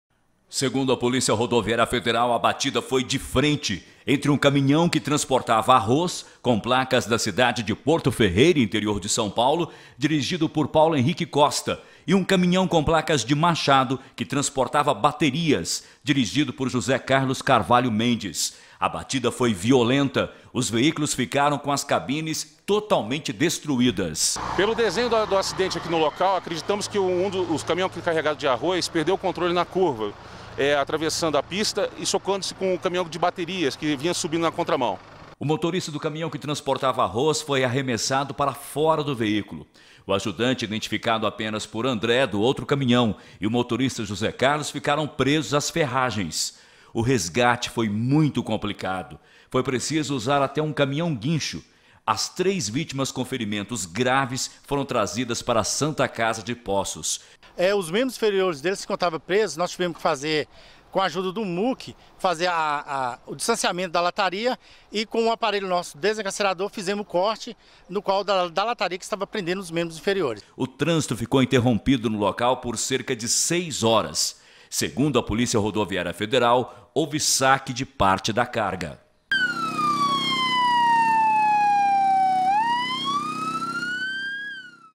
BATIDA ENTRE 2 CAMINHÕES
Três pessoas ficaram feridas em um acidente nesta segunda-feira (23 fevereiro) na BR 146, entre Poços e Bandeira e Sul.